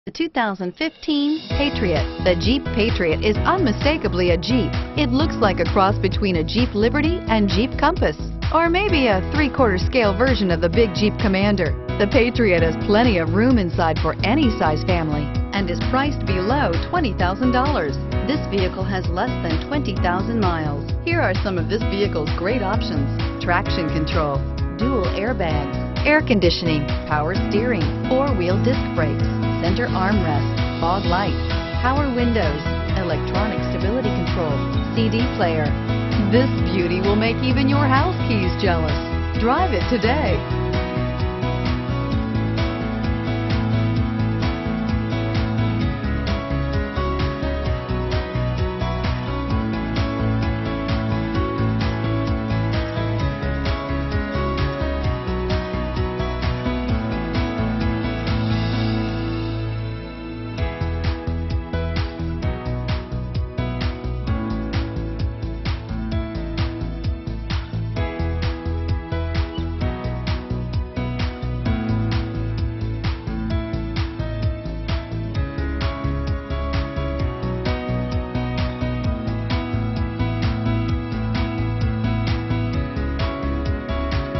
2015 Jeep Patriot St. Charles IL C2800
2015 Jeep Patriot

1611 E. Main St.

4WD. My! My! My! What a deal! Gasoline! This gorgeous 2015 Jeep Patriot is the SUV that you have been looking for. It only has 16k miles, meaning there's tons of room left on the odometer.